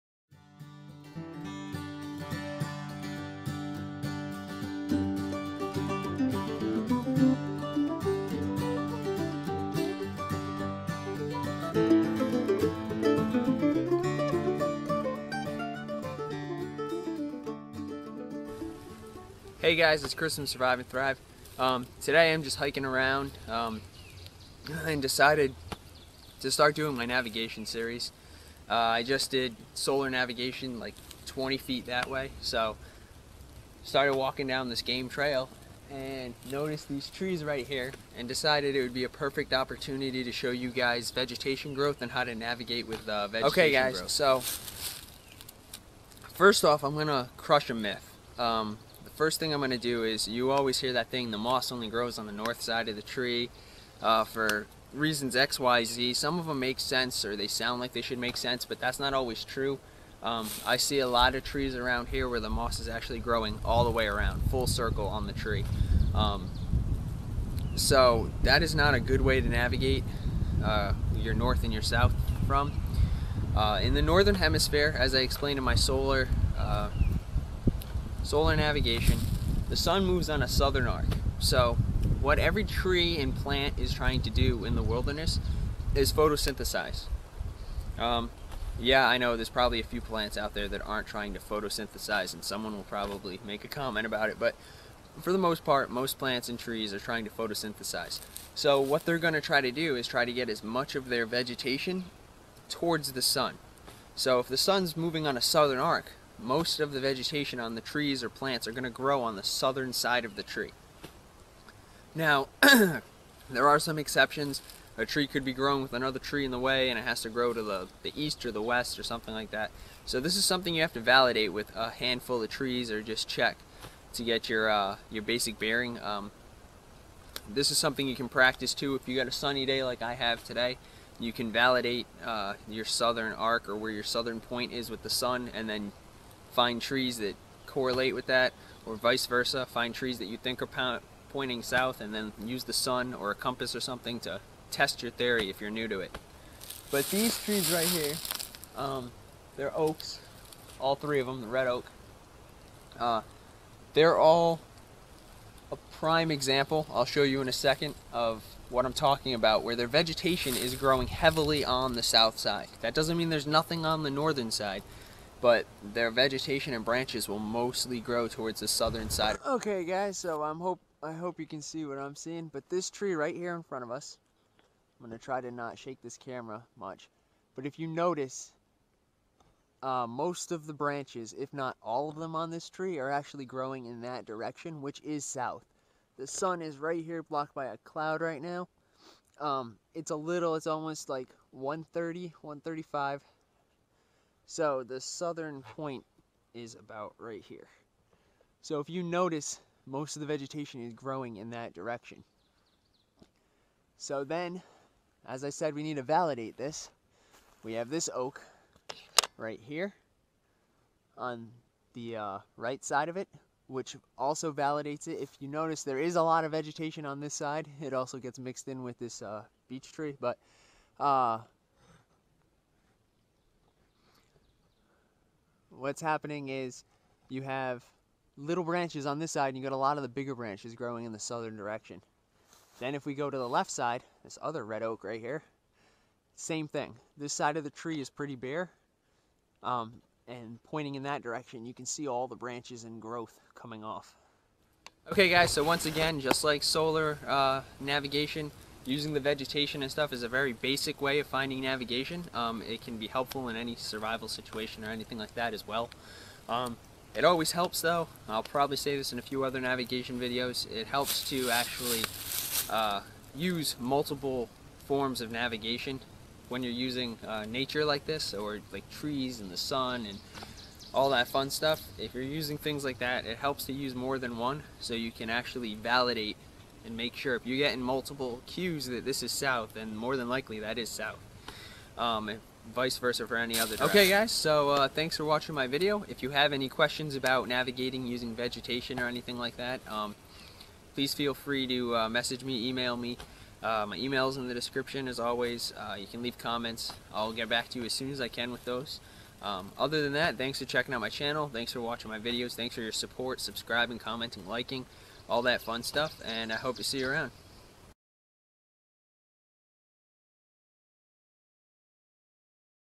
Navigation with vegetation
A simple navigation video on how to navigate using trees and surrounding vegetation.  Just like solar or lunar navigation this is a very basic form of navigation.  Whether in a pinch or just for fun it can be a neat way to orient yourself as you go.
Anything I missed?  Something you want to see? Have a question?